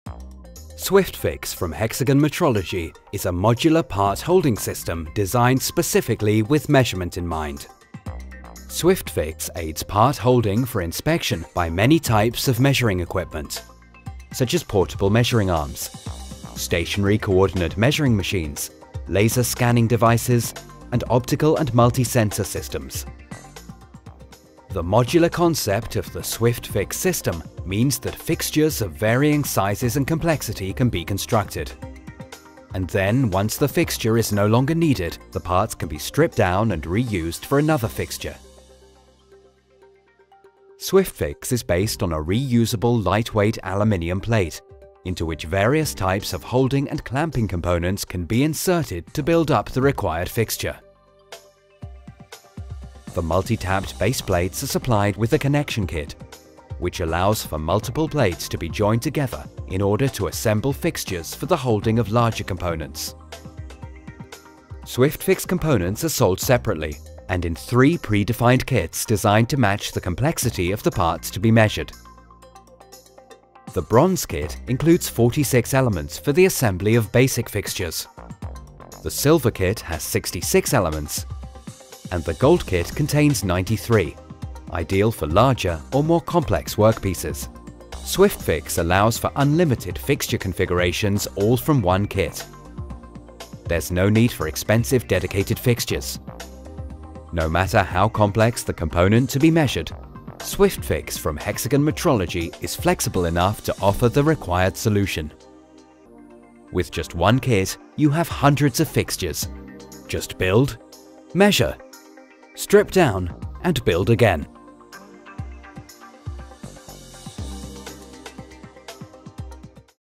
Swift-Fix CMM Modular Fixture System
The Swift-Fix modular CMM fixturing system from Hexagon Metrology offers a reusable, reconfigurable system with modular threaded baseplates that enables you to build an unlimited number of fixtures to hold parts for inspection with bridge CMMs and portable CMMs. Just build, measure, strip down, and build again.